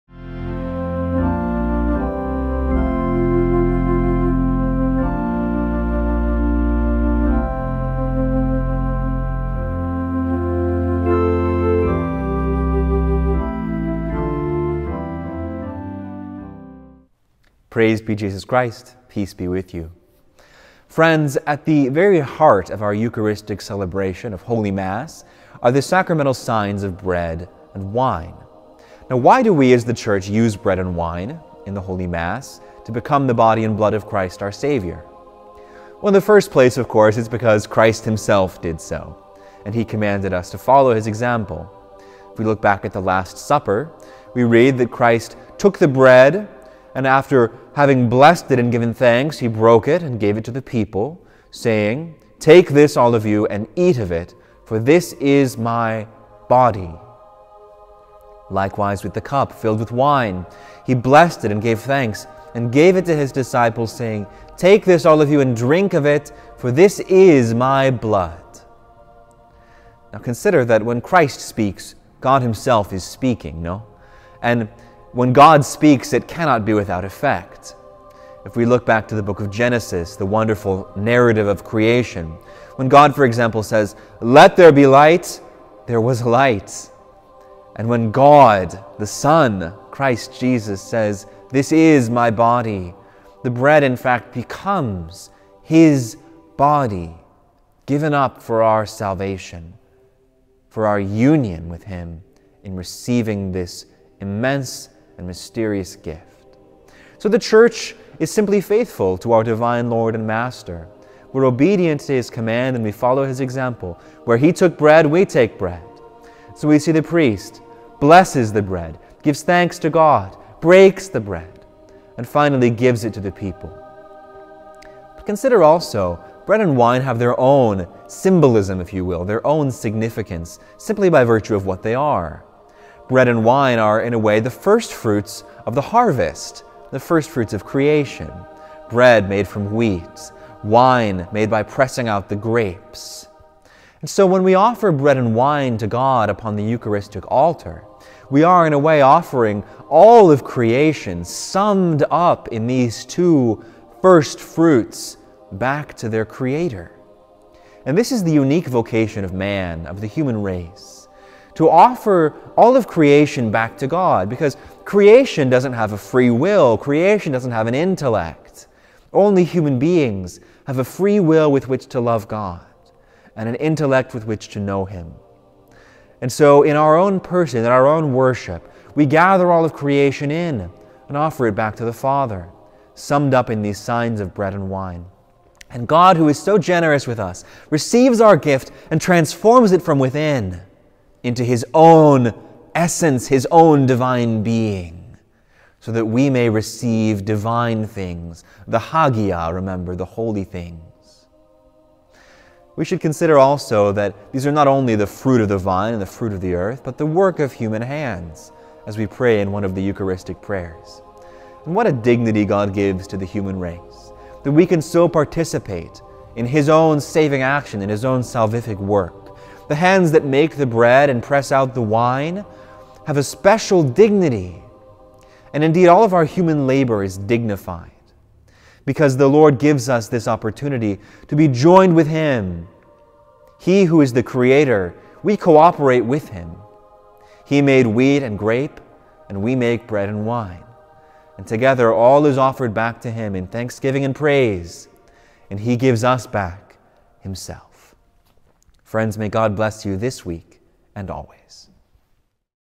RECEIVE | Eucharistic Catechesis (CCC 1333) | St. Mary's Catholic Church
Come and join seminarian Matthew Knight in this short weekly catechesis on the Holy Eucharist, and deepen your faith in the "source and summit" of our Catholic life. "Whoever eats my flesh and drinks my blood has eternal life" (Jn 6:54). Taste and see that the Lord is good!

This week's topic comes from the Catechism of the Catholic Church:

"At the heart of the Eucharistic celebration are the bread and wine that, by the words of Christ and the invocation of the Holy Spirit, become Christ's Body and Blood. Faithful to the Lord's command the Church continues to do, in his memory and until his glorious return, what he did on the eve of his Passion: 'He took bread. . . .' 'He took the cup filled with wine. . . .' The signs of bread and wine become, in a way surpassing understanding, the Body and Blood of Christ; they continue also to signify the goodness of creation. Thus in the Offertory we give thanks to the Creator for bread and wine, fruit of the 'work of human hands,' but above all as 'fruit of the earth' and 'of the vine' - gifts of the Creator. The Church sees in the gesture of the king-priest Melchizedek, who 'brought out bread and wine,' a prefiguring of her own offering" (CCC 1333).

RECEIVE is a production of Matthew Knight and St. Mary's Catholic Church, Eugene, Oregon.